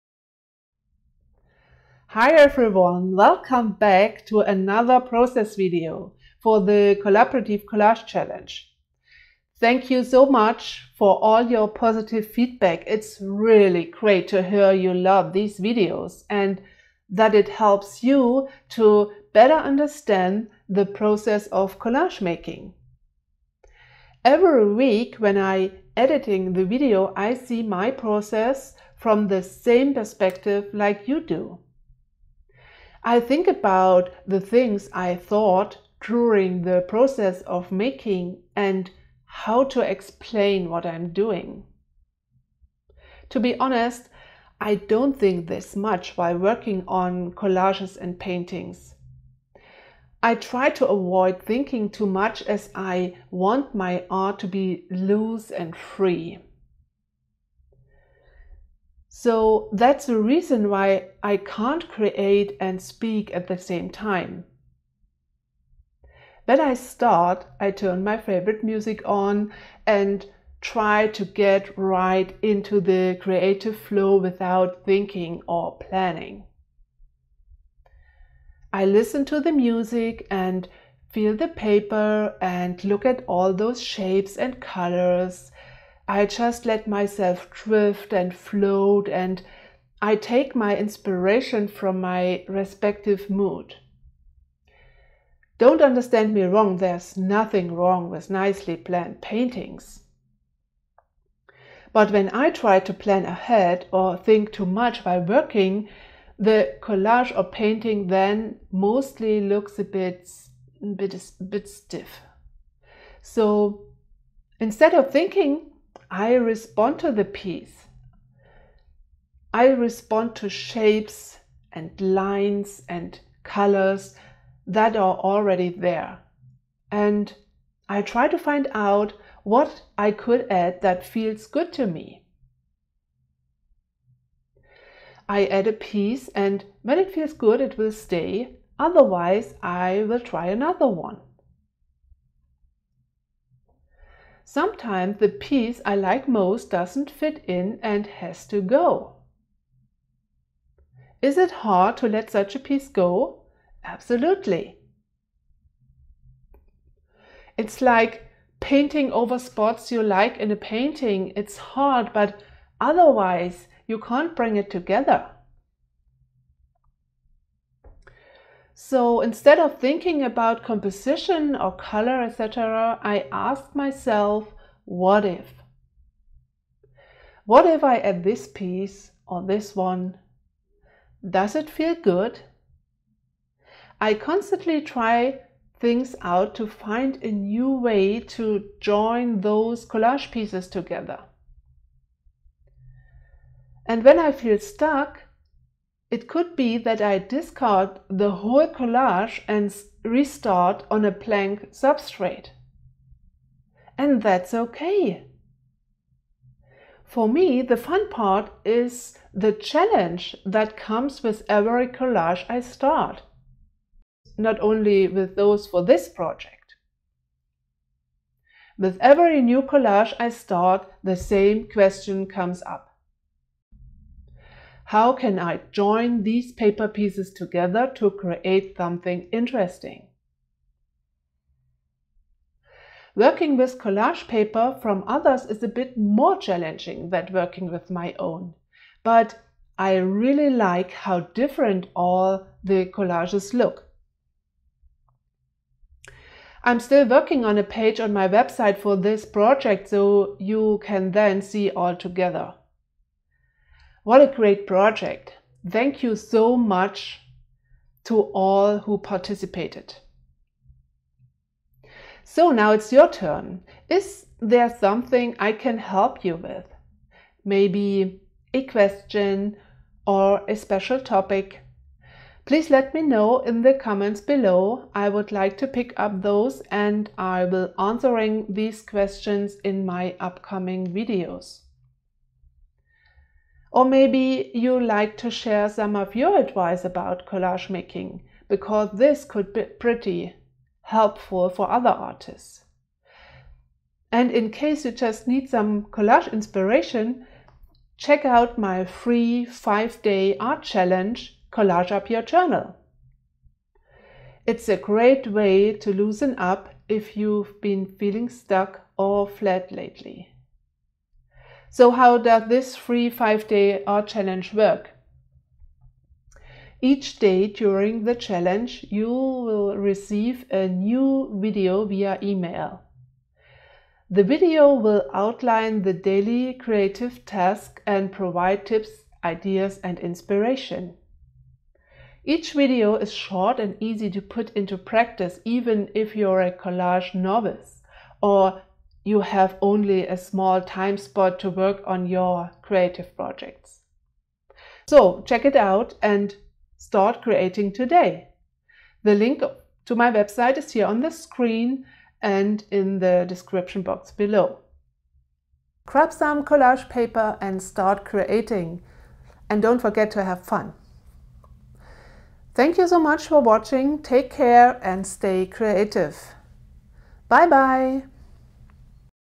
Collage With Me! - and let me know your questions about collage making.
Welcome to another process for the Collaborative Collage Challenge!
Thank you so much for all your positive feedback.
It's really great to hear you love these videos and that it helps you to better understand the process of collage making.

To support you in your creative process of collage making I would like to hear your questions about collage making.
Is there something I can help you with?

Each week, I share tips, tricks, advice and inspiration to help you getting back into the creative flow 🙌